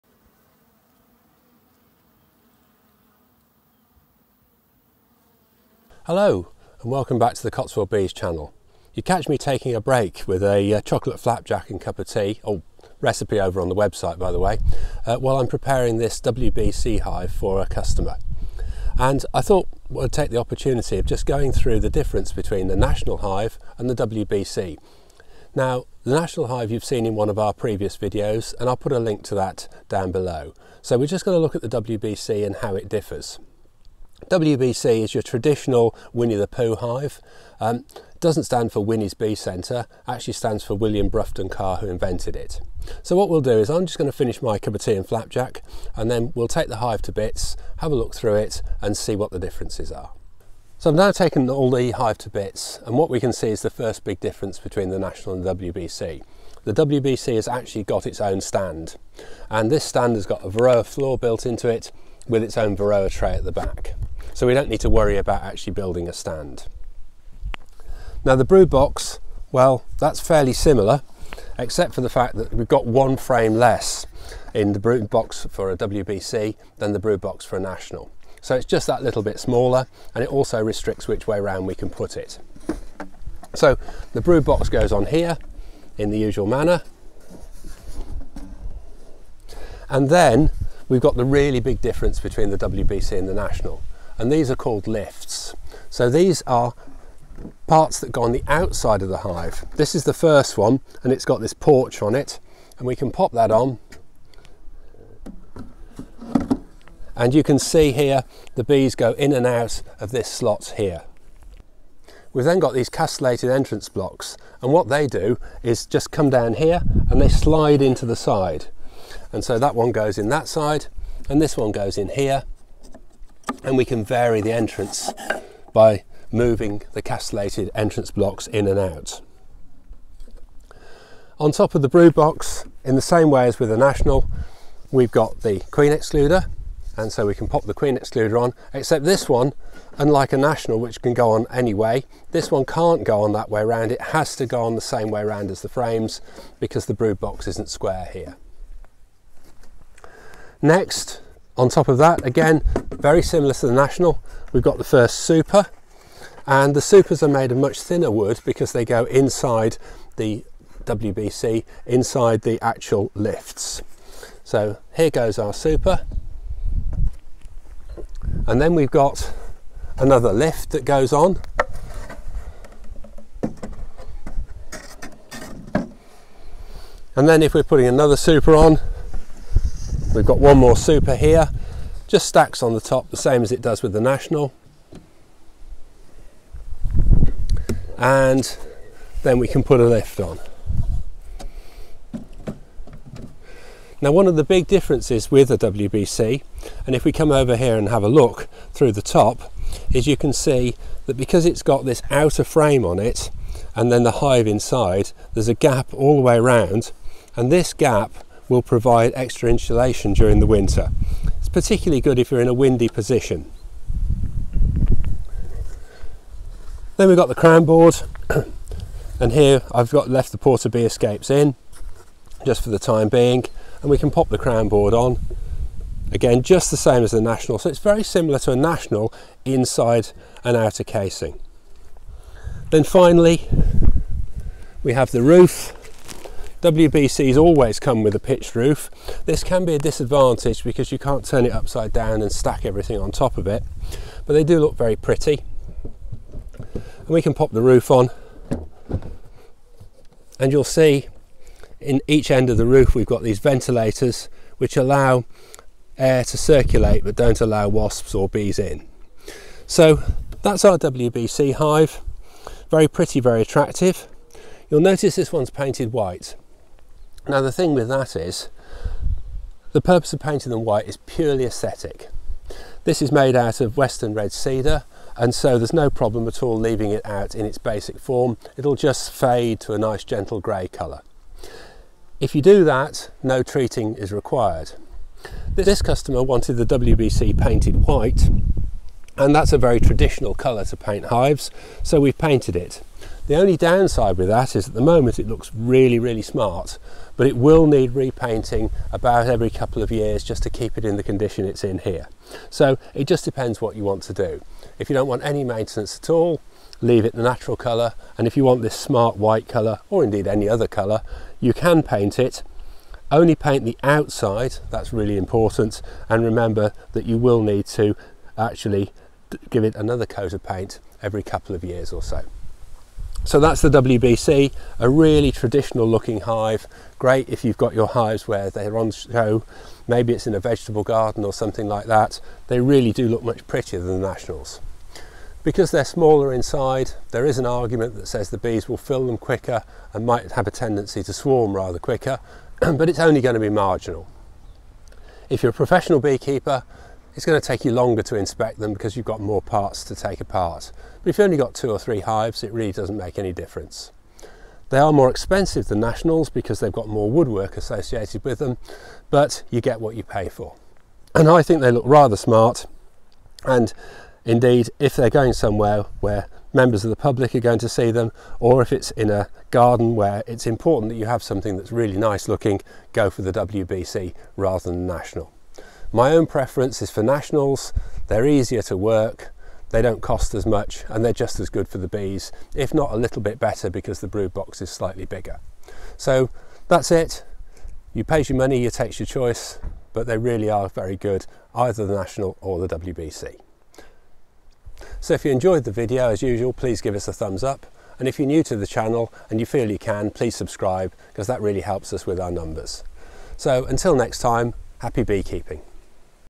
The WBC Hive - With Cotswold Bees
In this video Chris looks at the advantages and disadvantages of the WBC hive and compares it with the National Hive. Nationals and WBC’s are the most widely used hives by hobby beekeepers in the UK.

If you want to know more about the a National Hive please have a look at our Setting Up The Apiary Video

If you want to know more about beekeeping why not try our online courses